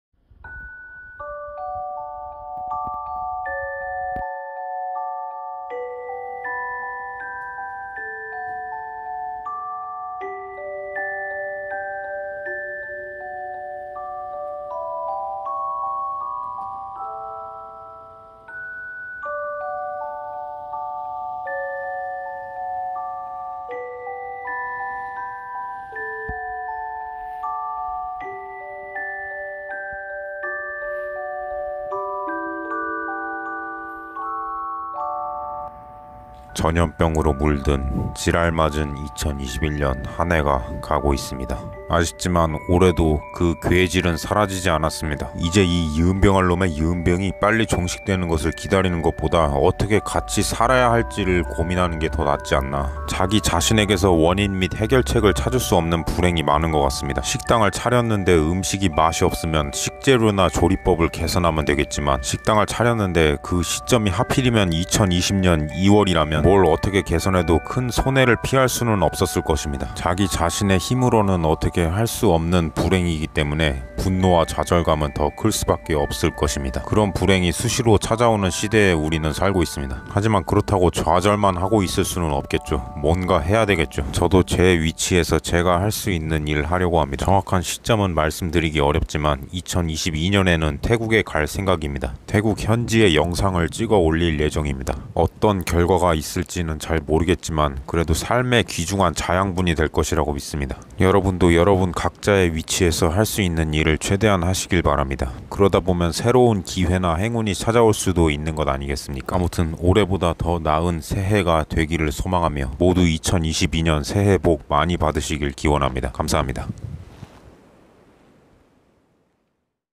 연말인사 & 2022년 태국에 다시 갈 계획
2022년에는 태국에 다시 가서 영상을 올려보겠습니다
모두 2022년 새해 복 많이 받으십시오